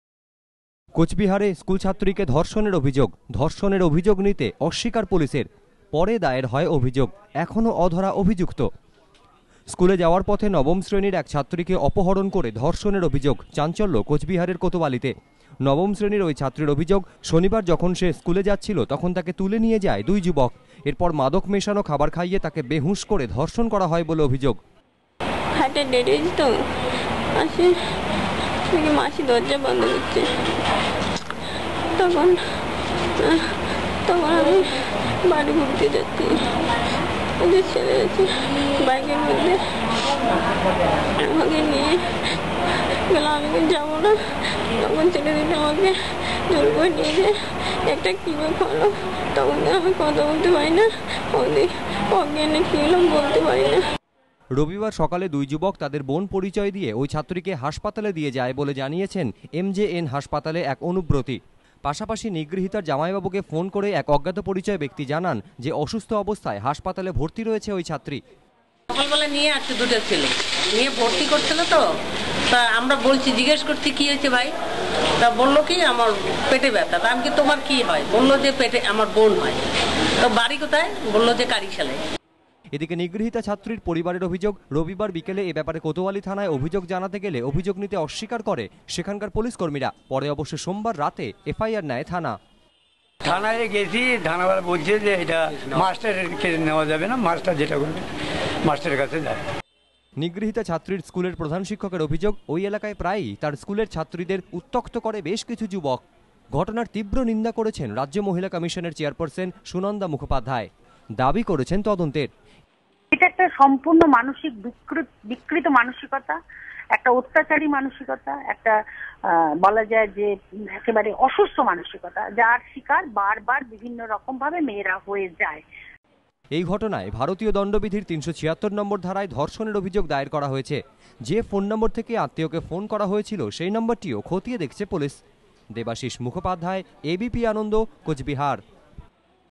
coochbihar schoolgirl rape
class nine girl student allegedly abducted and raped by miscreants in coochbihar